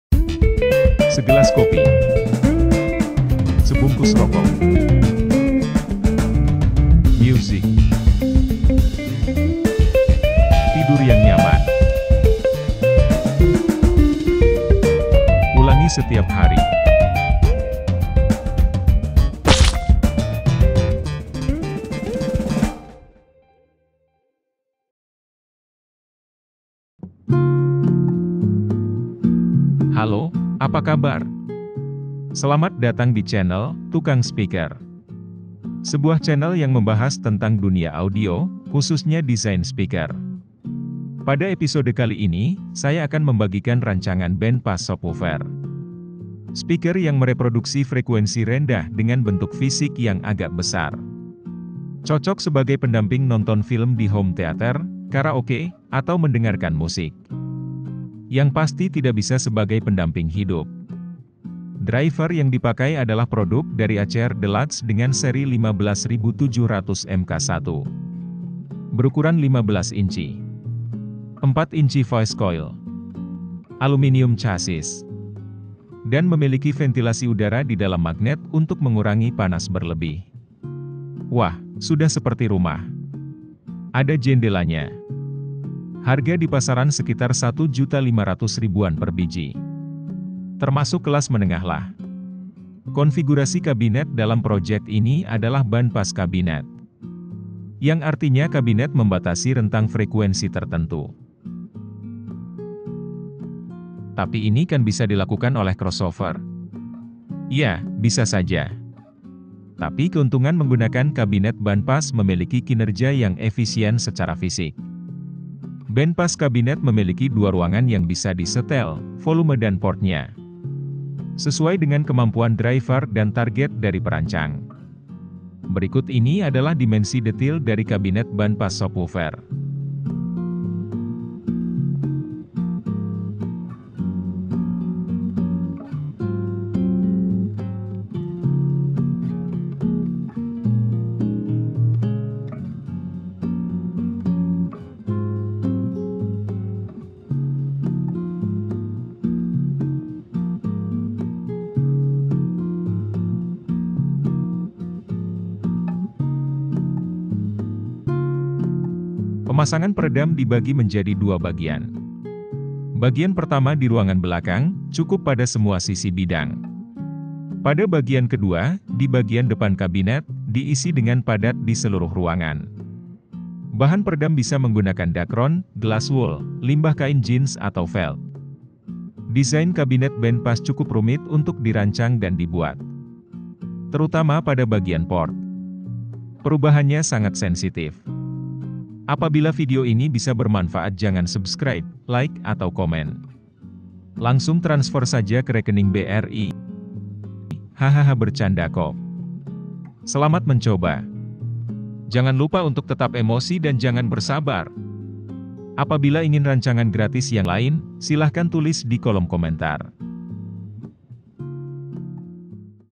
Membuat Sendiri Subwoofer Bandpass 15 Inch ACR Deluxe (Simulation)
Untuk Design Khusus, silahkan langsung meluncur : wa.link/7dwgks